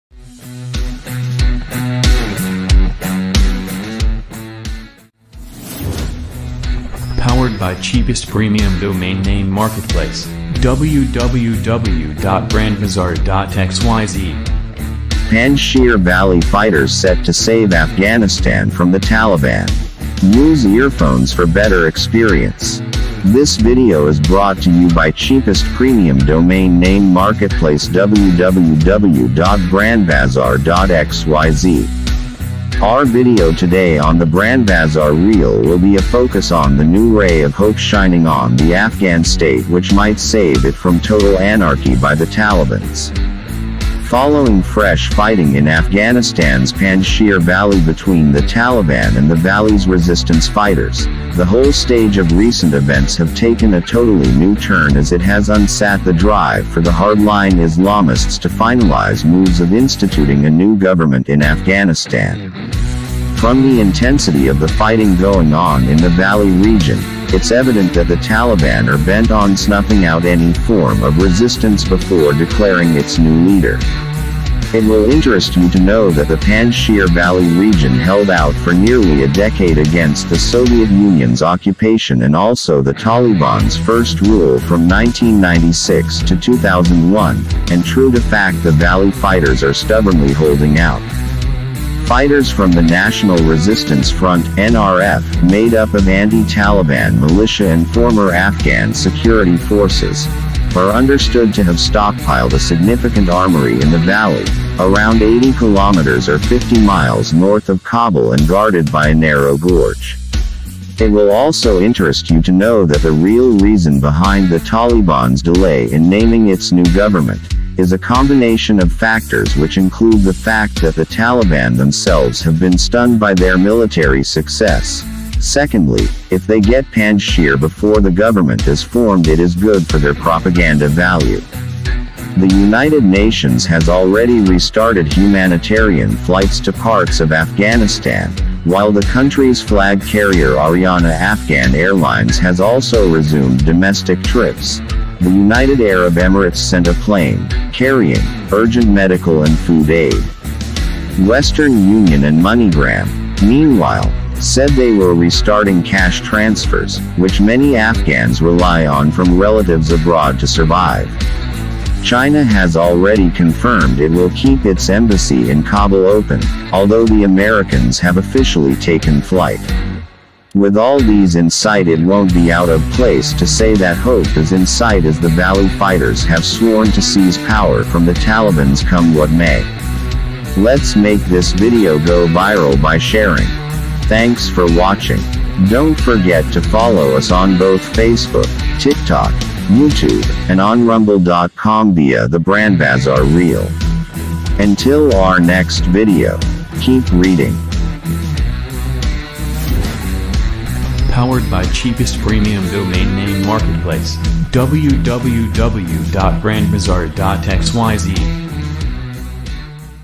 PANJSHIR VALLEY FIGHTERS SET TO SAVE AFGHANISTAN FROM THE TALIBAN
Cheapest Premium Domain Name Marketplace

Our video today on The BrandBazaar Reel will be a focus on the new ray of hope shining on the Afghan state which might save it from total anarchy by the Talibans.

Following fresh fighting in Afghanistan's Panjshir Valley between the Taliban and the valley's resistance fighters, the whole stage of recent events have taken a totally new turn as it has unsat the drive for the hardline islamists to finalize moves of instituting a new government in Afghanistan.

From the intensity of the fighting going on in the valley region, it's evident that the Taliban are bent on snuffing out any form of resistance before declaring it's new leader. It will interest you to know that the Panjshir valley region held out for nearly a decade against the Soviet Union’s occupation and also the Taliban’s first rule from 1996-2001, and true to fact the valley fighters are stubbornly holding out.

Fighters from the National Resistance Front (NRF) — made up of anti-Taliban militia and former Afghan security forces - are understood to have stockpiled a significant armoury in the valley, around 80 kilometres (50 miles) north of Kabul and guarded by a narrow gorge.

It will also interest you to know that the real reason behind the Taliban's delay in naming it's new government, is a combination of factors which include the fact that the Taliban themselves have been stunned by their military success; secondly, if they get Panjshir before the government is formed it is good for their propaganda value.

The United Nations has already restarted humanitarian flights to parts of Afghanistan, while the country’s flag carrier Ariana Afghan Airlines has also resumed domestic trips; the United Arab Emirates sent a plane carrying - urgent medical and food aid. Western Union and Moneygram, meanwhile, said they were restarting cash transfers, which many Afghans rely on from relatives abroad to survive. China has already confirmed it will keep its embassy in Kabul open, although the Americans have officially taken flight. With all these in sight it won't be out of place to say that hope is in sight as the valley fighters have sworn to seize power from the Talibans come what may.